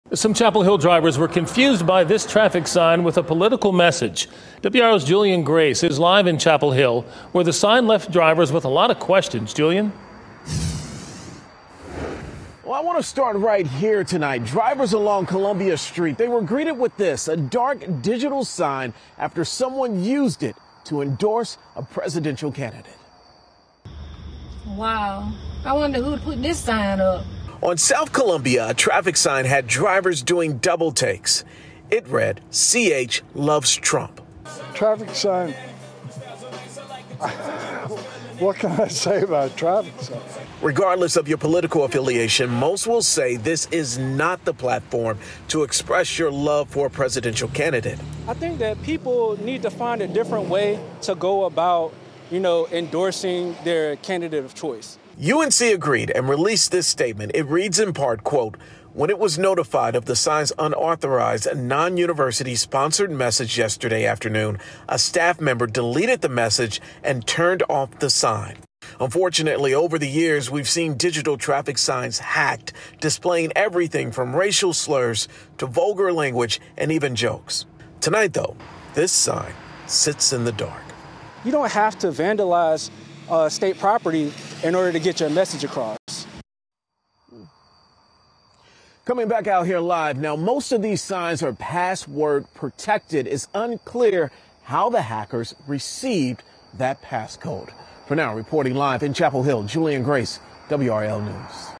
NCDOT sign in Chapel Hill changed to 'CH loves Trump'
A digital traffic sign in South Columbia, Chapel Hill, caused a stir after it was used to display a message endorsing a presidential candidate.